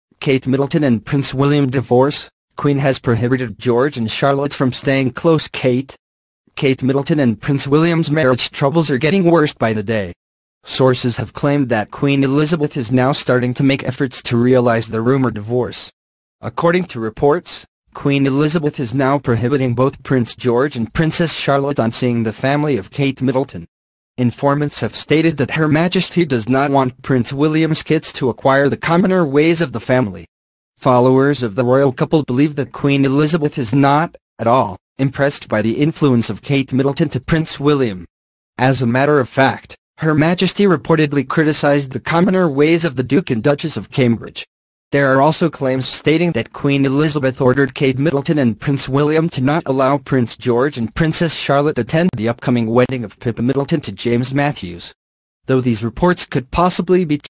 Kate Middleton and Prince William divorce, Queen has prohibited George and Charlotte from staying
Kate Middleton and Prince William divorce, Queen has prohibited George and Charlotte from staying close kate.

Kate Middleton and Prince William's marriage troubles are getting worse by the day. Sources have claimed that Queen Elizabeth is now starting to make efforts to realize the rumored divorce.

According to reports, Queen Elizabeth is now prohibiting both Prince George and Princess Charlotte on seeing the family of Kate Middleton. Informants have stated that Her Majesty does not want Prince William's kids to acquire the commoner ways of the family.